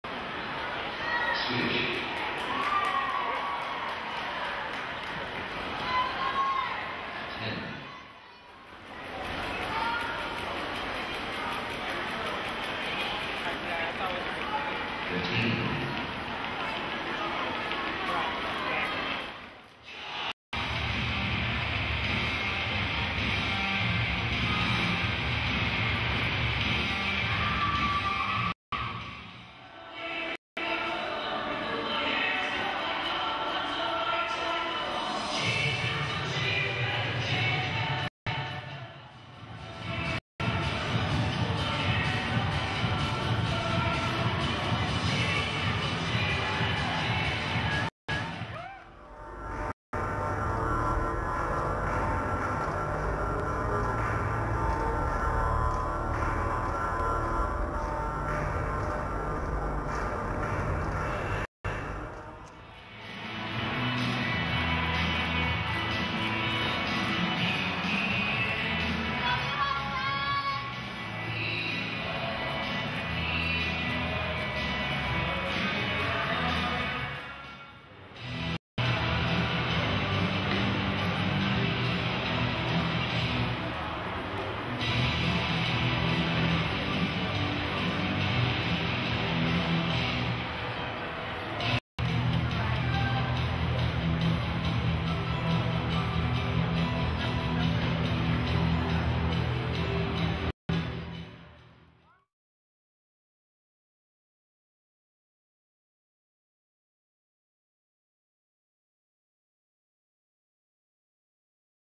International Jump Rope Competition
The Cary YMCA Super Skippers (Amanda, Melissa, Amy, Lauren, and Anna) took the overall Gold medal (JR Female team: age 12-14) at the Maple Leaf International Rope Skipping competition in Toronto (July 2006). They received a gold or silver medal in six of the eight events.  for lots more jump rope videos.